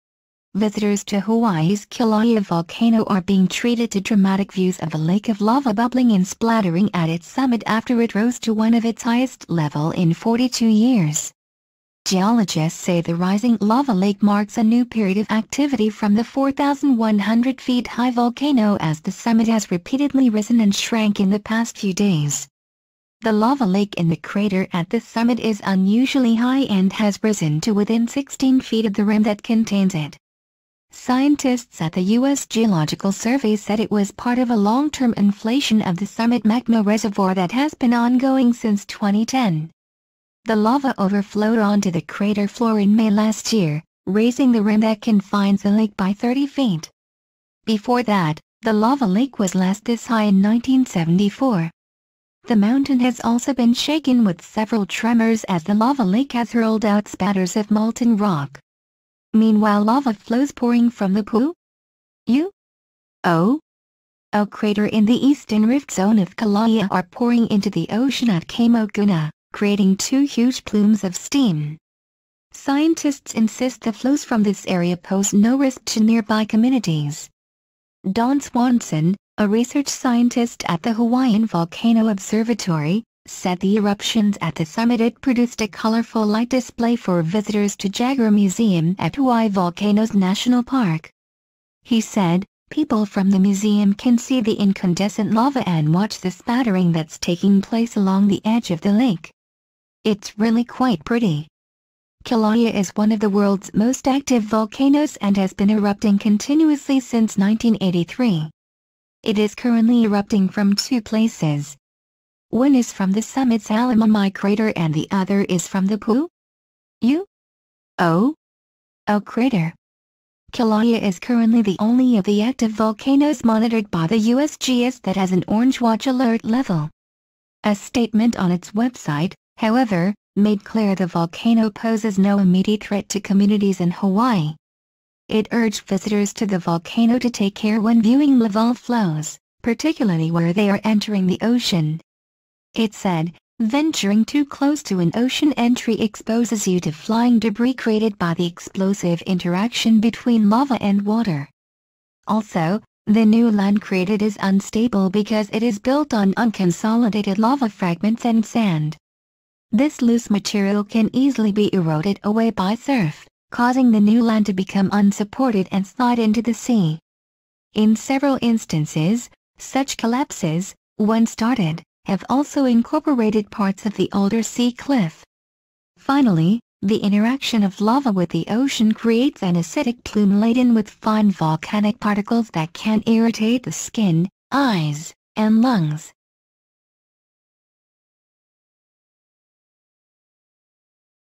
Is Hawaii's Kilauea volcano about to blow?
Is Hawaii's Kilauea volcano about to blow? Lava lake at summit of world's most active volcano is rising
Visitors to Hawaii's Kilauea volcano are being treated to dramatic views of a lake of lava bubbling and splattering at its summit after it rose to one of its highest level in 42 years.
Geologists say the rising lava lake marks a new period of activity from the 4,100 feet high volcano as the summit has repeatedly risen and shrank in the past few days.